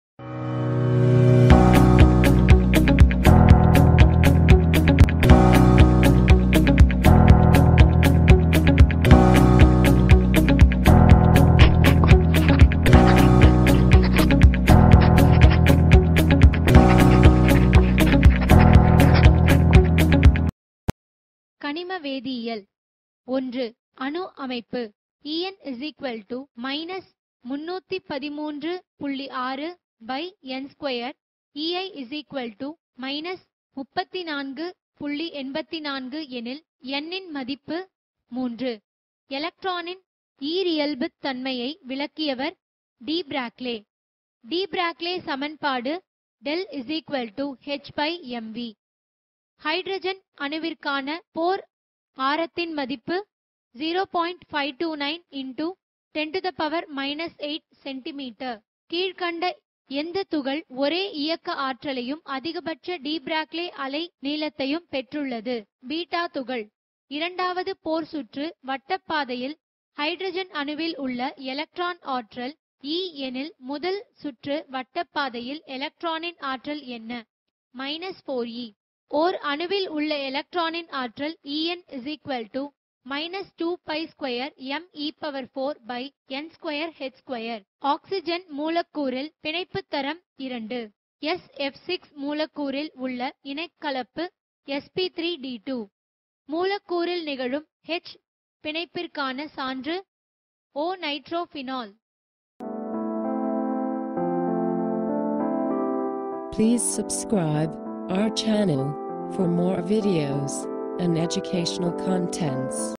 QBanca | Tamilnadu State Board | 12th Std | chemistry | Tamil Medium | Unit 1 | Part-1
QBanca.com presents Tamil medium Tamil nadu State Board 12th standard chemistry Subject questions - Part-1